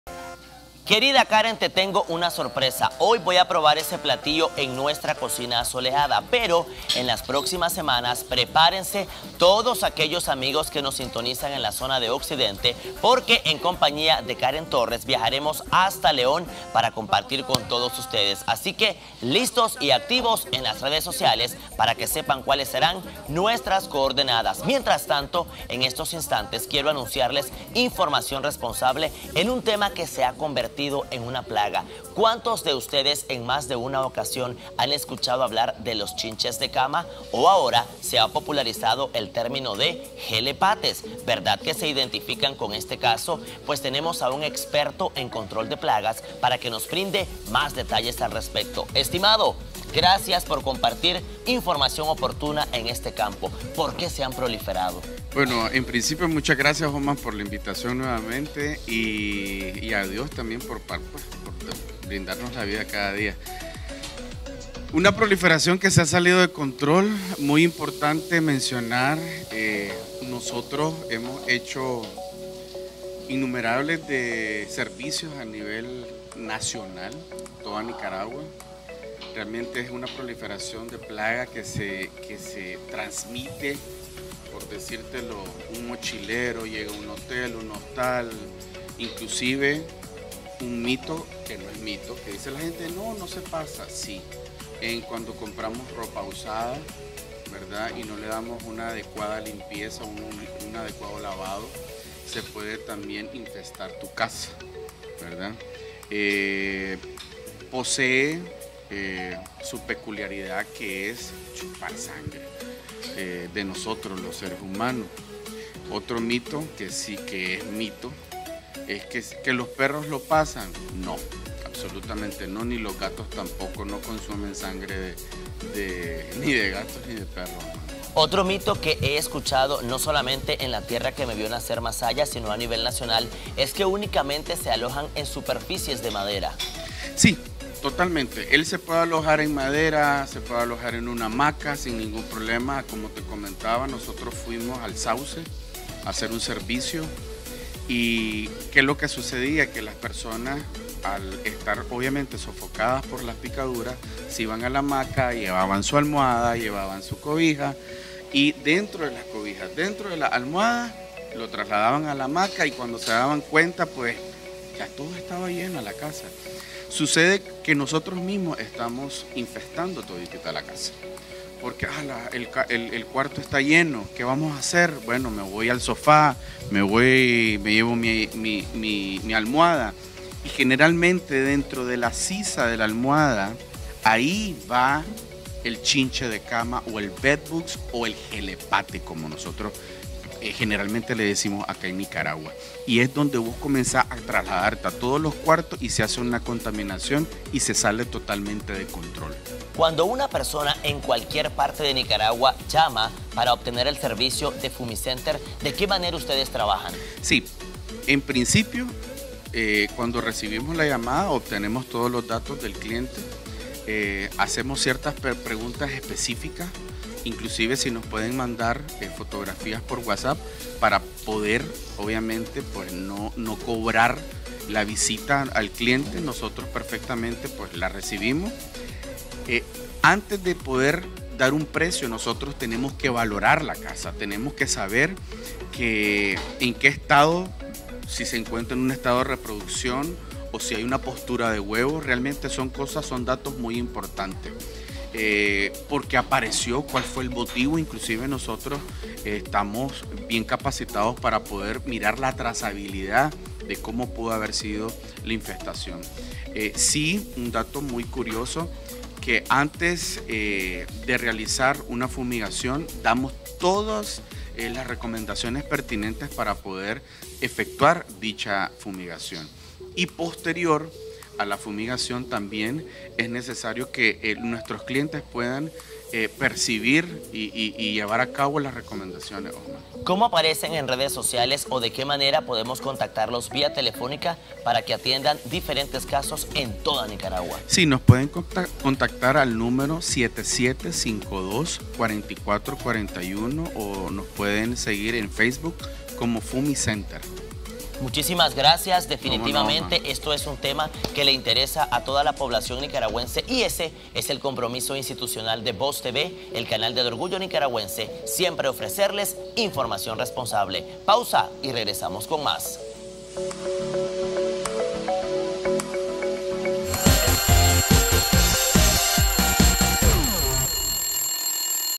¿Por qué aparecen las chinches de cama?
Jonathan Ortíz, gerente de Fumicenter, comparte en la siguiente entrevista datos importantes sobre las chinches de cama, también conocidas como “jelepates”, y los métodos que ofrece la empresa para controlar esta plaga.